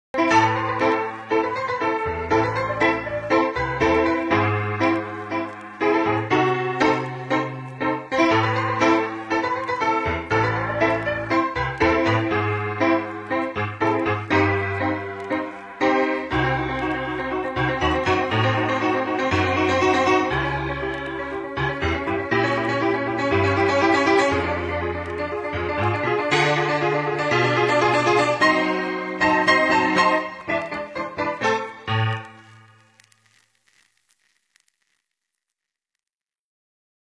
Jack The Ripper Advert
To celebrate our 40th anniversary, we're looking back in time.
Who knows who or what is lurking within the tunnels of Fort Amherst...
If you think you can survive, book your tickets now!

Guess who the lady in this advert is. 😜
Come visit us at Fort Amherst where we'll be dying to meet you. 😈